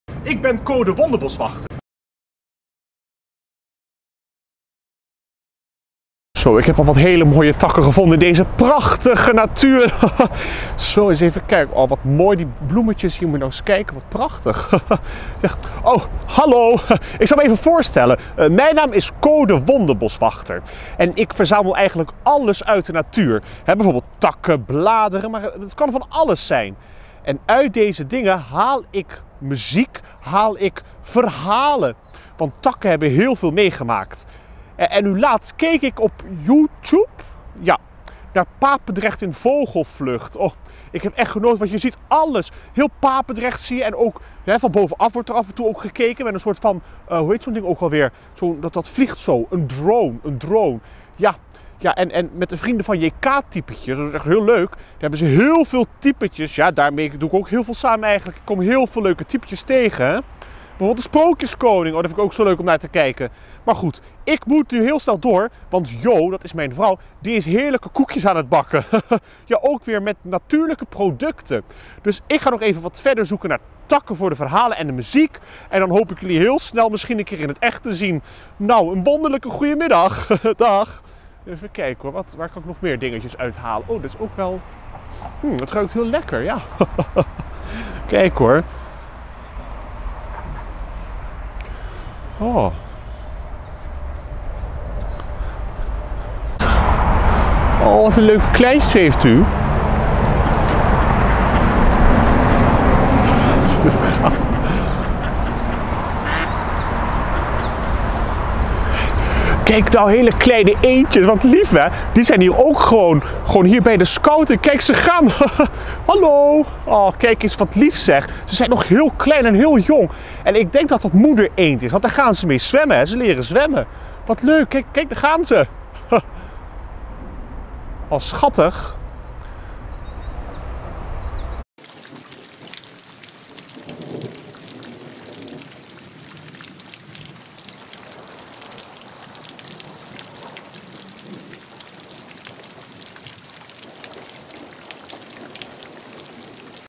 Co de Wonder Boswachter promotie film
we kijken mee met Co de wonder boswachter,
een van de typetjes van Jeffrey Kwakernaat